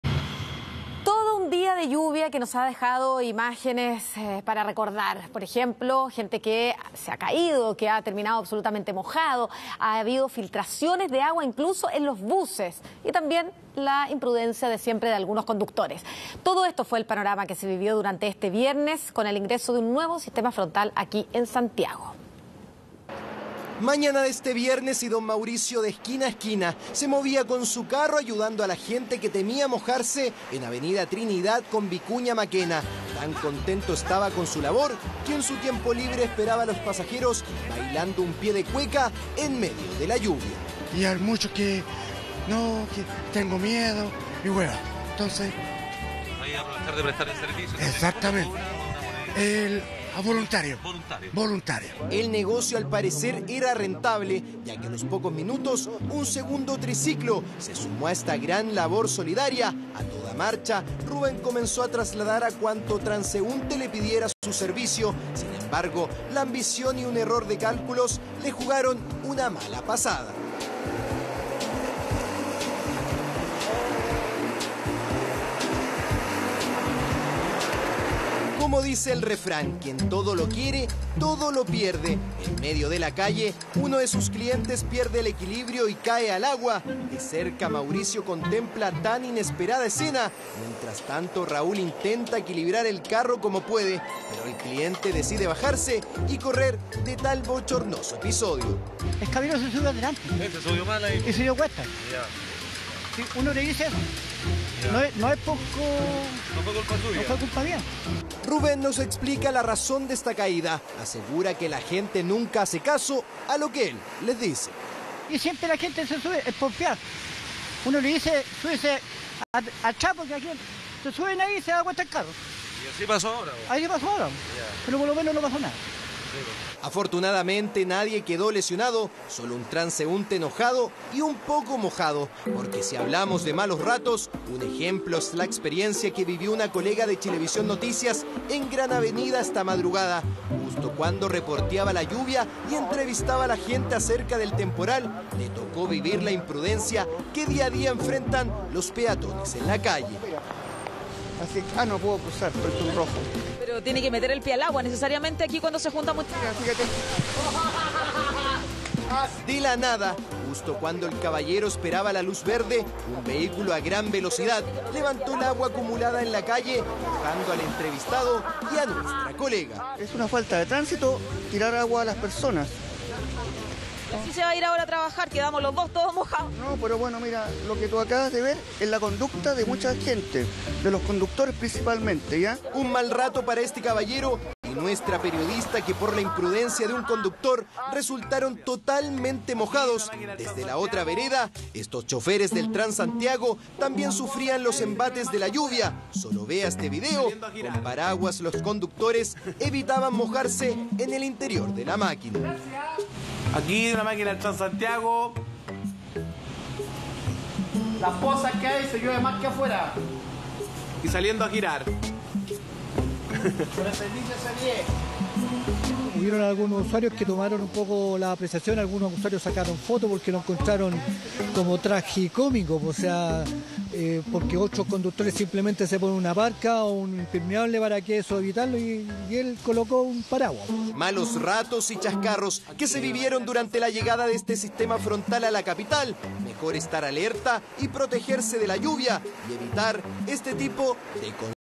Peatón cayó al agua desde triciclo que lo ayudaba a cruzar la calle - CHV Noticias
Un fuerte e intenso sistema frontal de lluvias y viento, ingresó este viernes a la capital, inundando varias calles.

Es por eso que de inmediato comenzaron a aparecer los triciclos, que ayudan a los peatones para cruzar las calles, las cuales se anegaron de agua.